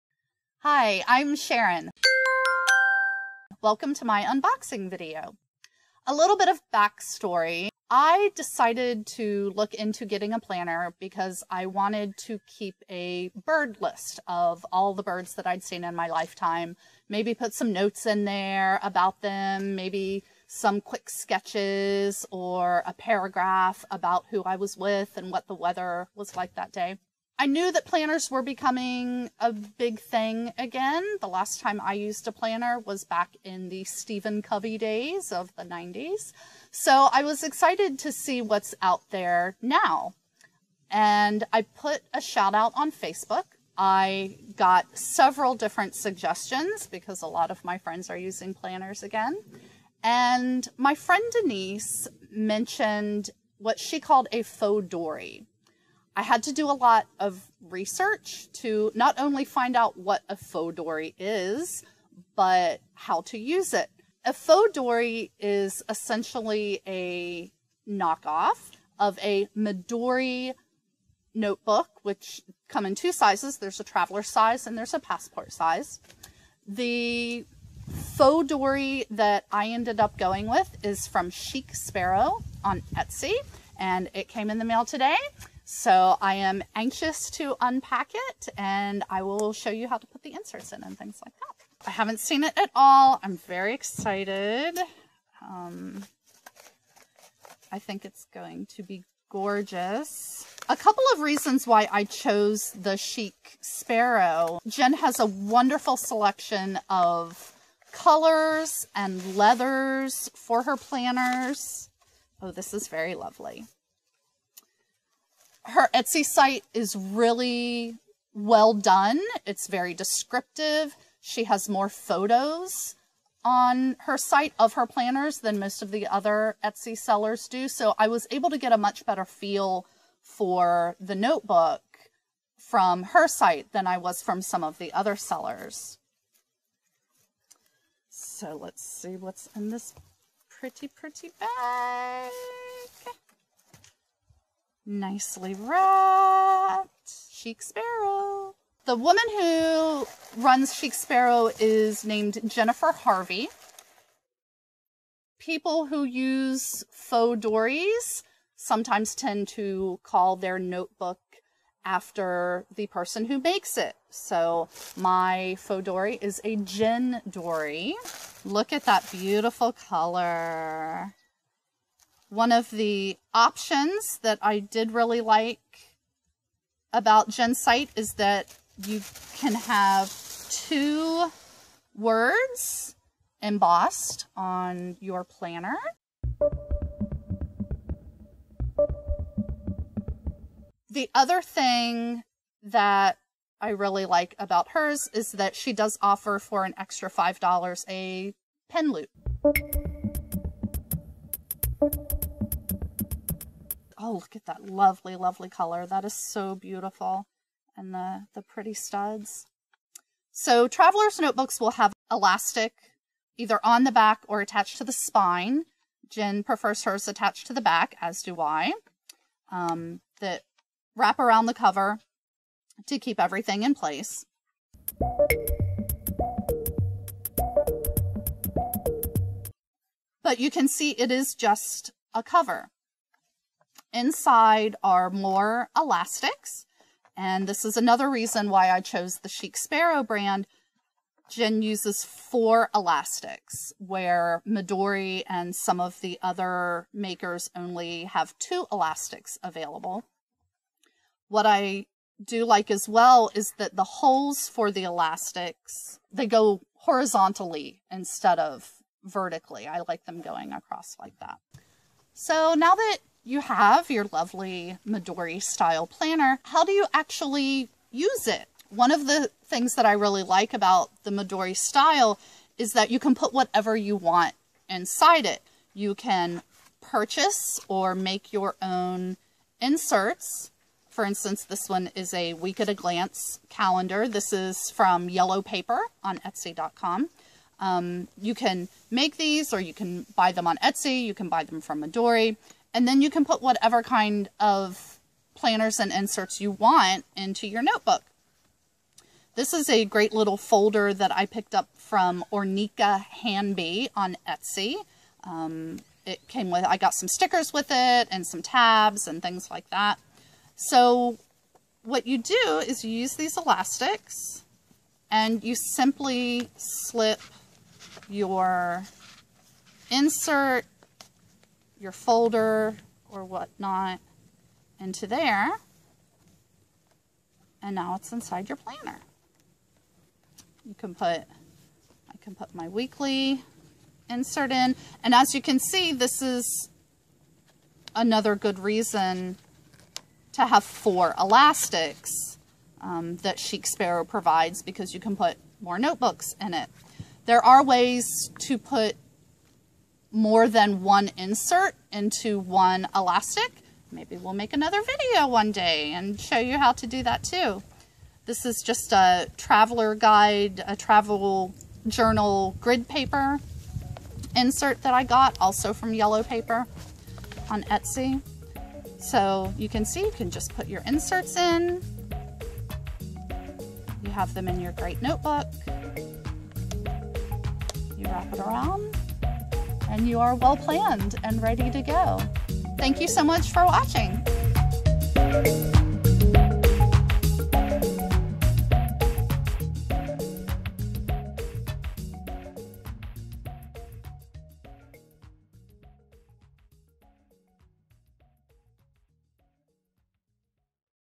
Jendori Unboxing
After I unbox my fauxdori planner cover, I'll show how inserts work in a Midori-style planner.